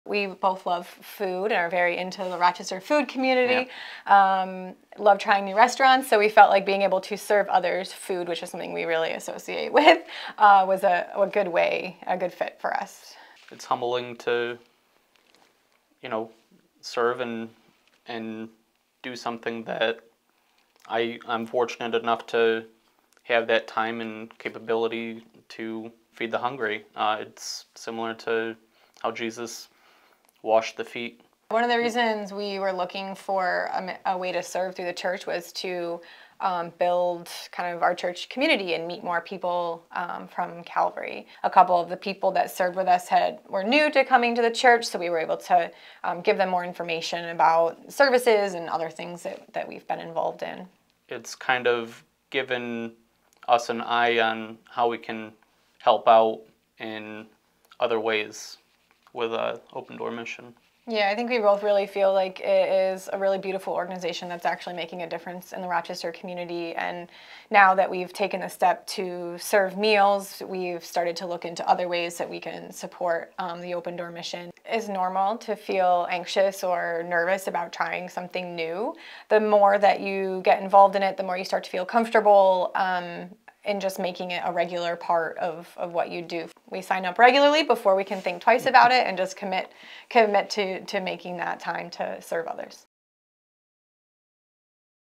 Serving at Open Door Mission
Casey and Emmaline share their story of serving at Calvary Assembly's Open Door Mission Outreach Team. Interested in getting involved?